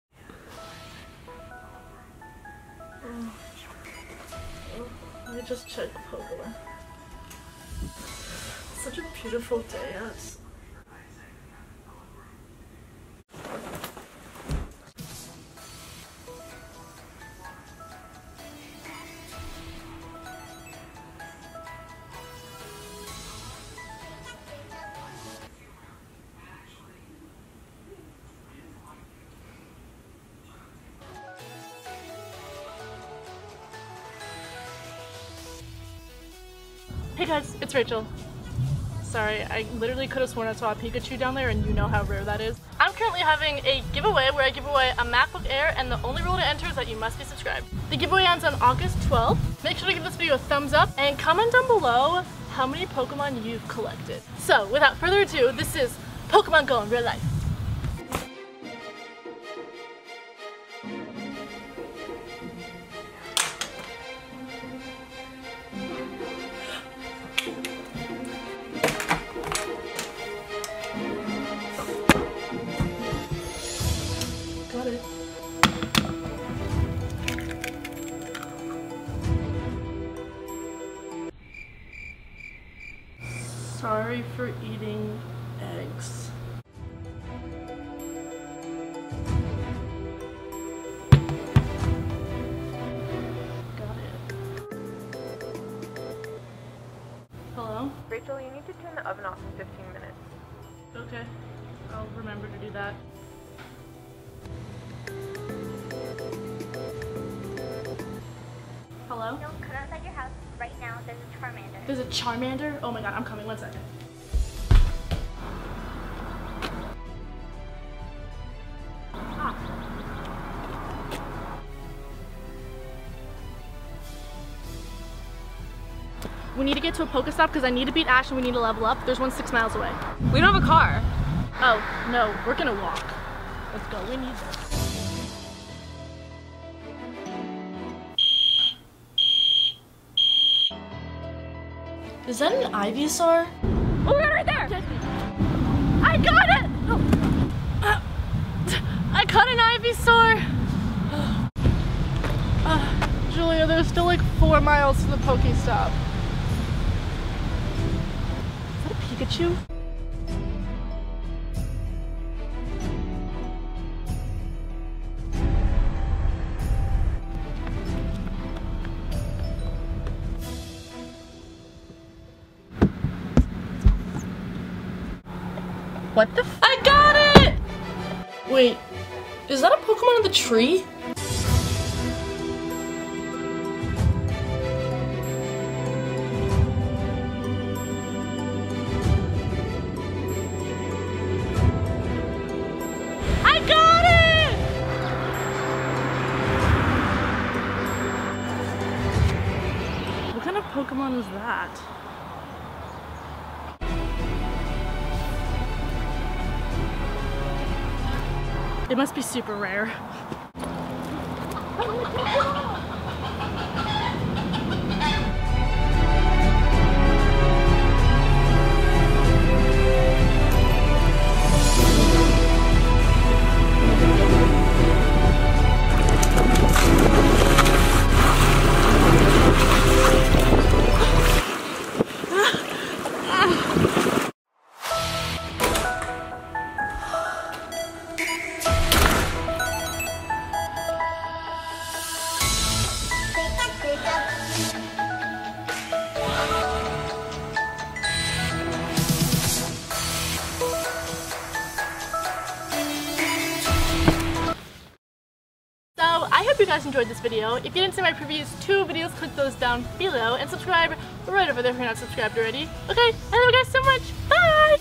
POKEMON GO IN REAL LIFE
Rachel Levin
Philadelphia, PA
19102
I decided to do a, pokemon go, video, because, pokemon go in real life, looked like a fun video to do!